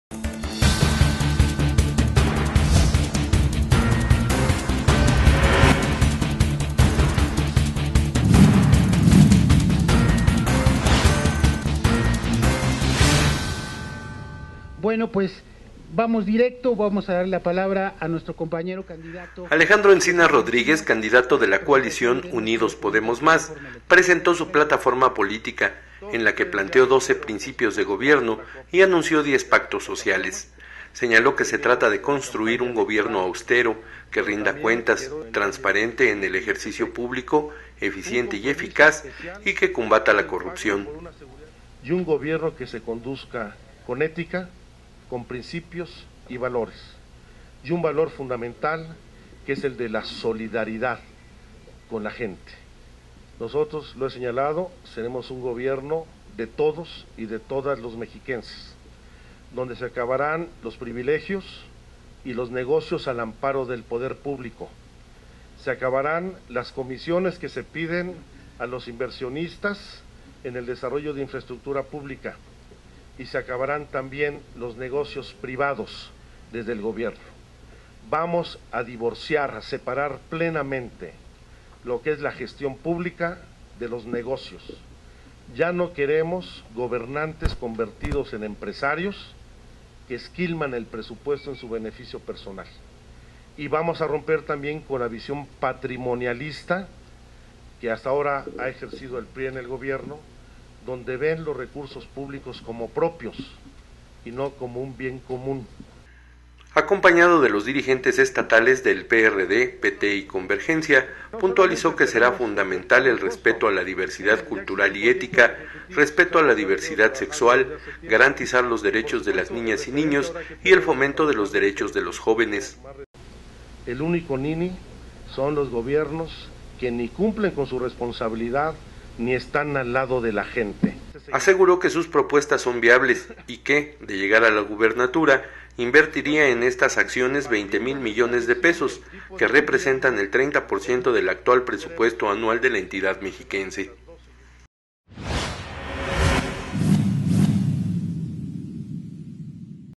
PRESENTA ALEJANDRO ENCINAS SU PLATAFORMA POLITICA
TOLUCA, México.- Alejandro Encinas Rodríguez, candidato de la coalición de izquierda Unidos podemos mas, presento su plataforma para la construcción de un gobierno que califico de austero, que rinda cuentas, transparente en el ejercicio publico, eficiente y eficaz, y que combata la corrupción.